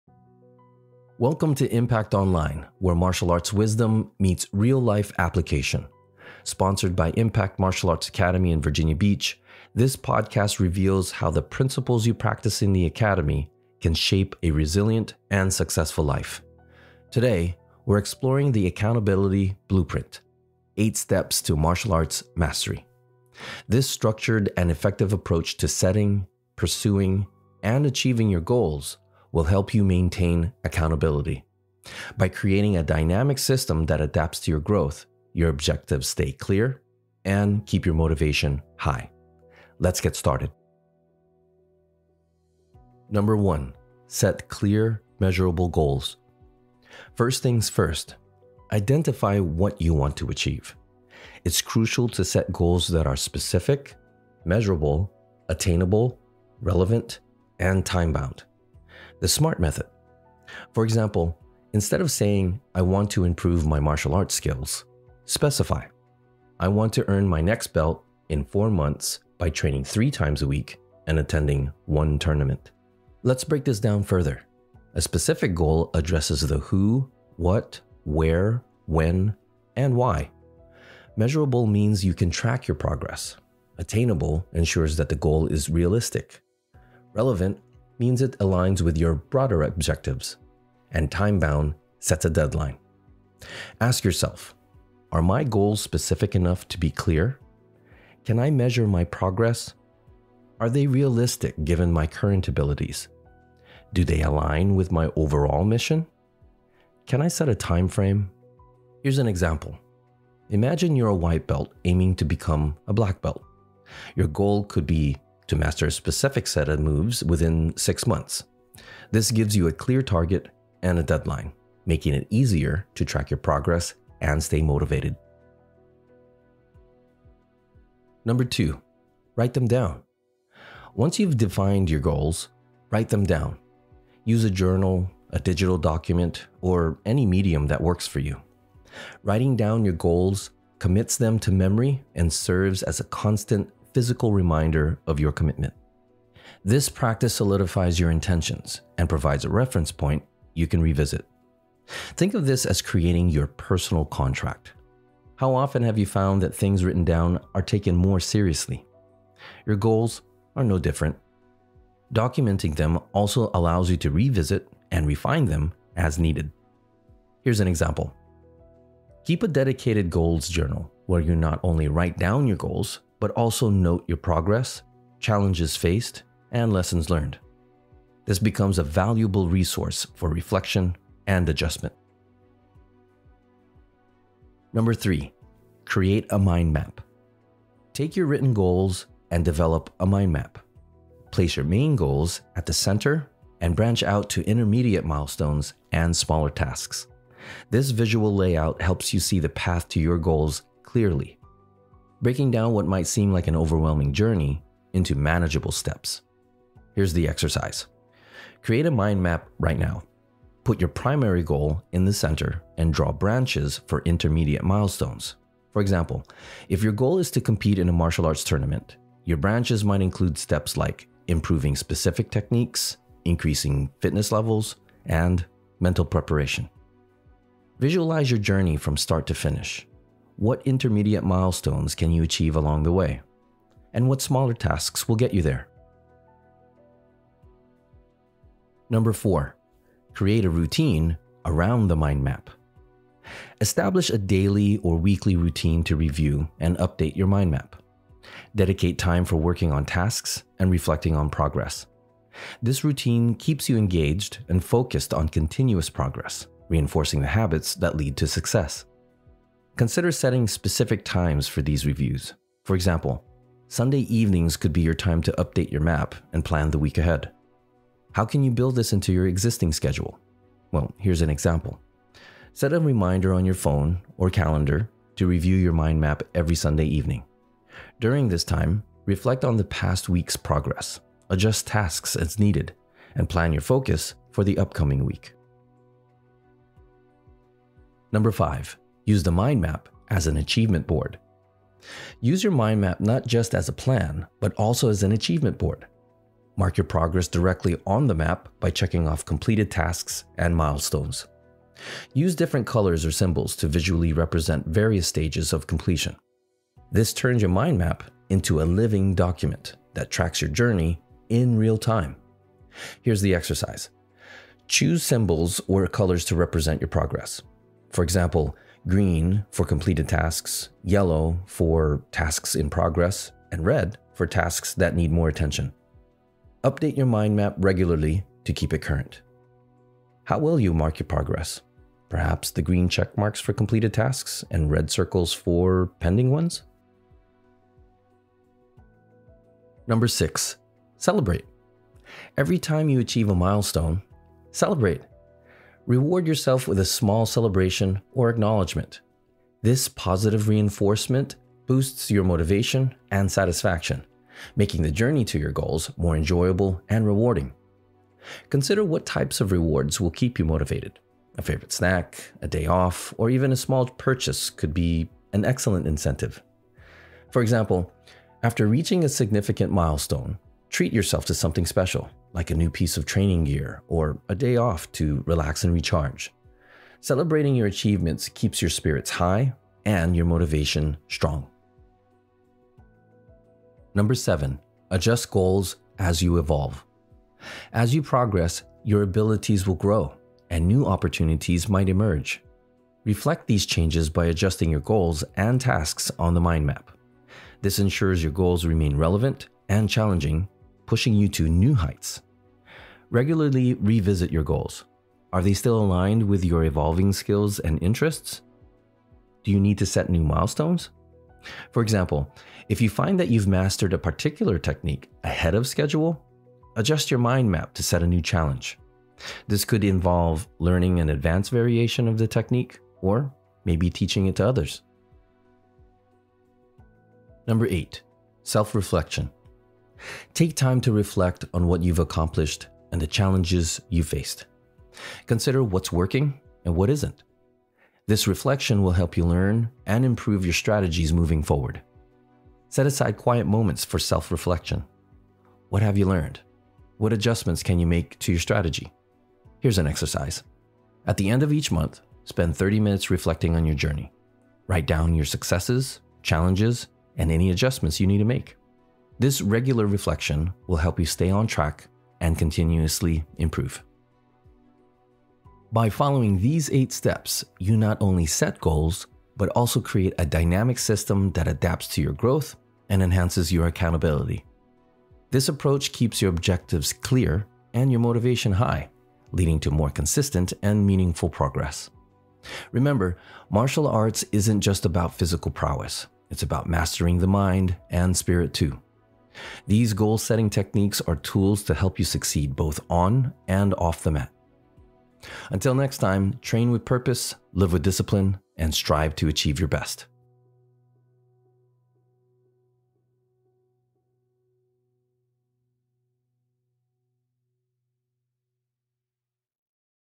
EP8: The Accountability Blueprint: 8 Steps to Martial Arts Mastery
This structured and effective approach to setting, pursuing, and achieving your goals will help you maintaining accountability. By creating a dynamic system that adapts to your growth, your objectives can stay clear and keep your motivation high.